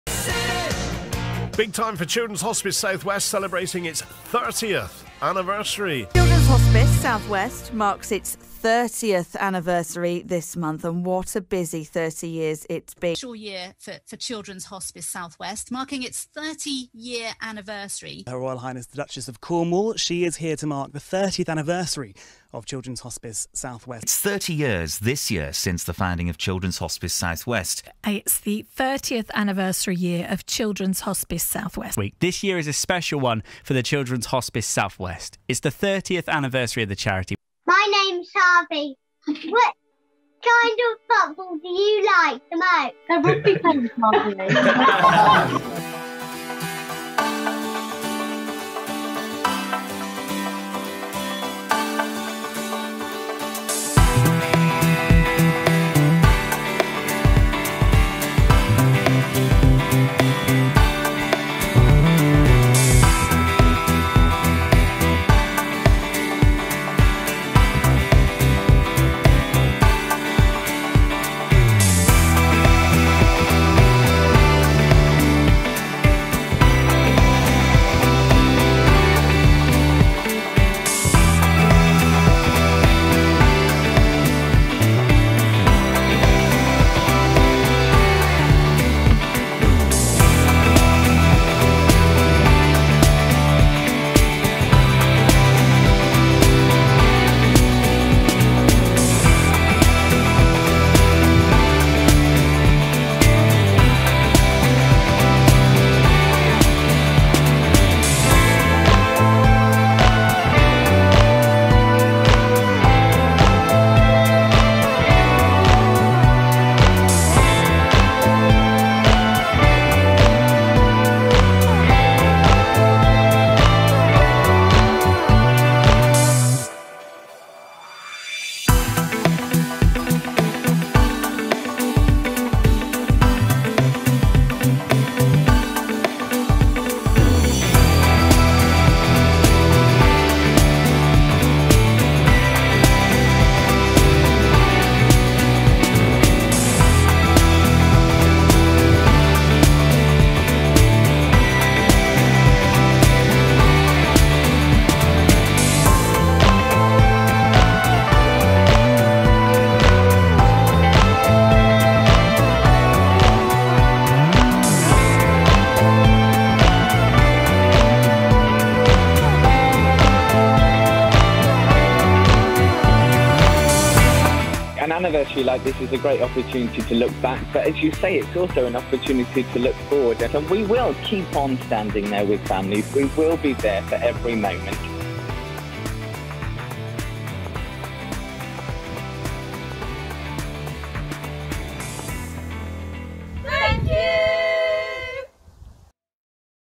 A snapshot from 2021
In a year when some things have felt impossible it's heart warming to look back and celebrate what has been possible - this video is just a snapshot from the last 12 months.
Thank you to everyone who has been a part of our 30th anniversary celebratory year, we look forward to continuing the journey in 2022.